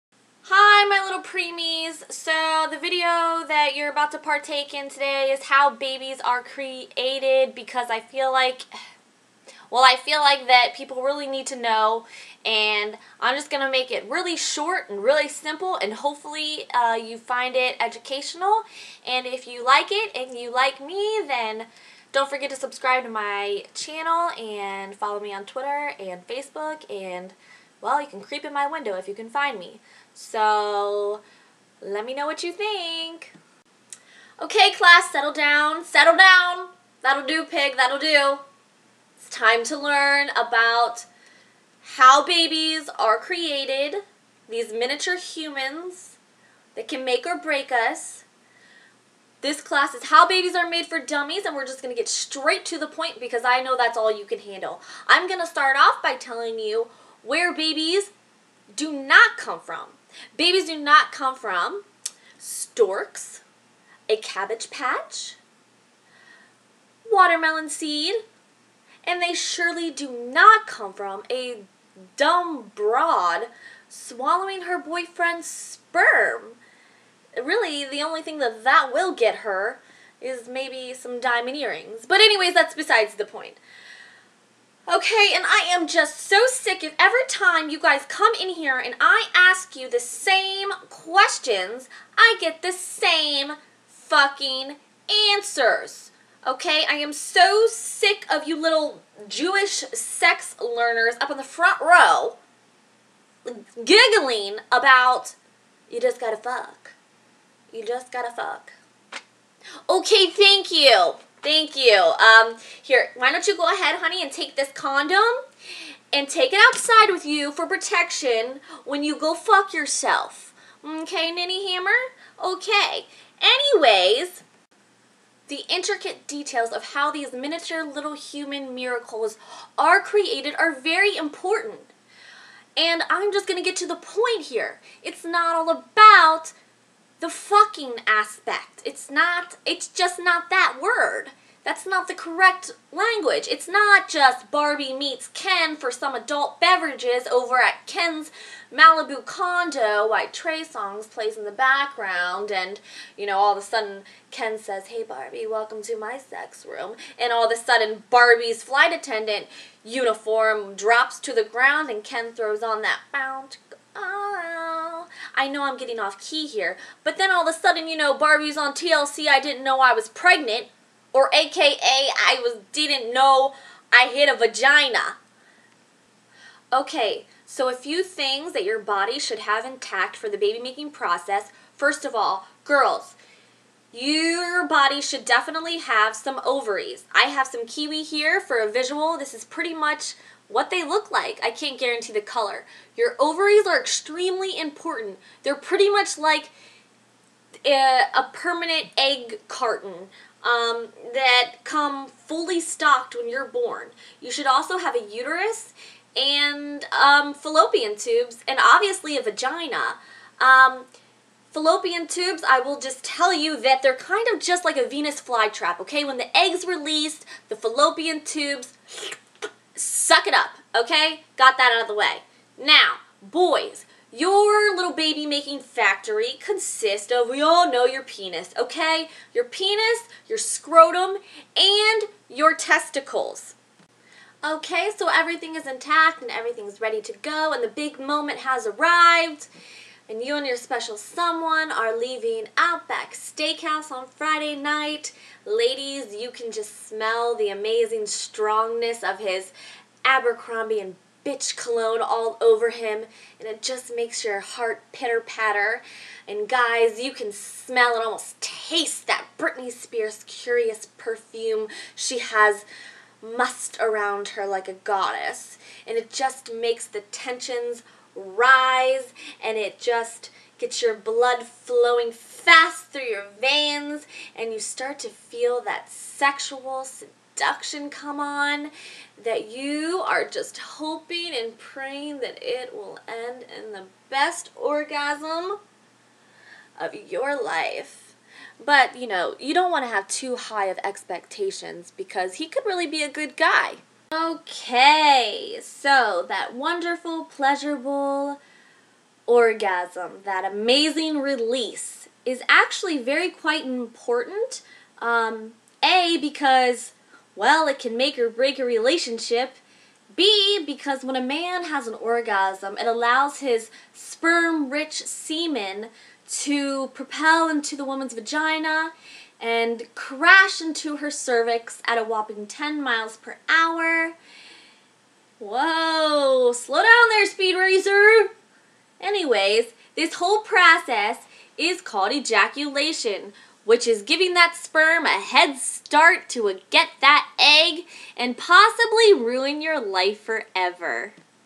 How babies are created for dummies
in making this video, i am well aware that most humans (not all) know how to have sex... in that being said this is just a quick over view of what is going on in the human body (totally condensed) when the magic happens :)